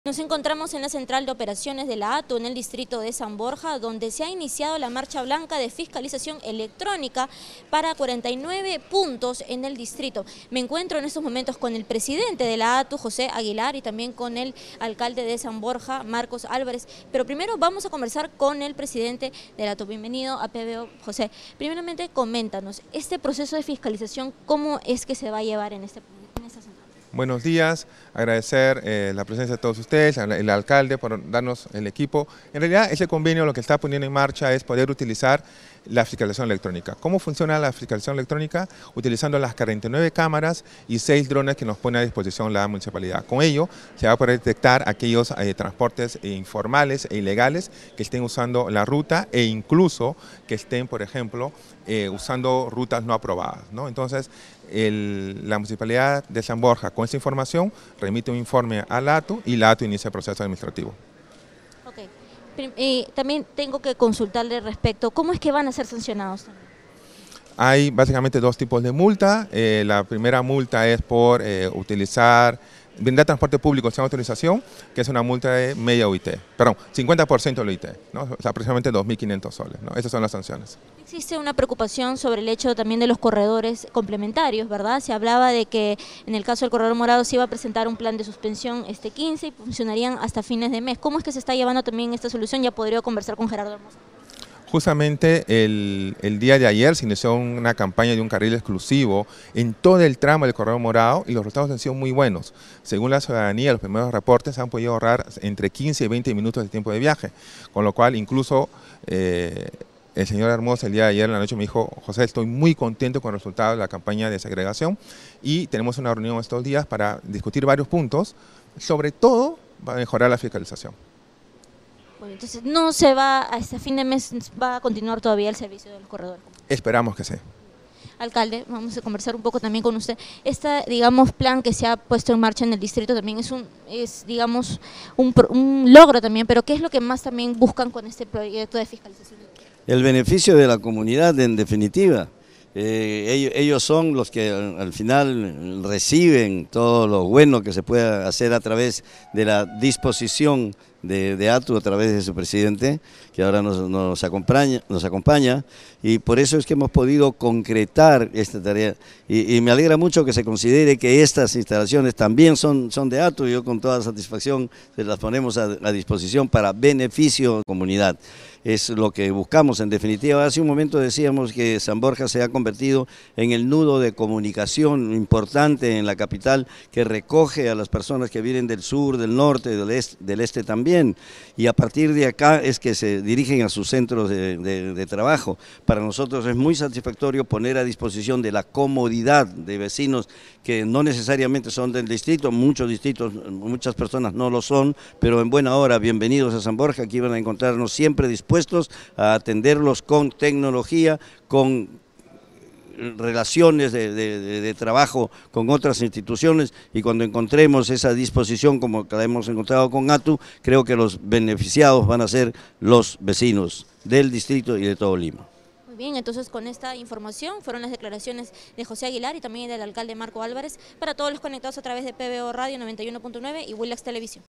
PBO | ATU da inicio a marcha blanca de fiscalización electrónica para combatir transporte informal
PBO
🌍 pbo.pe
📻 91.9 FM ¡La radio con fe!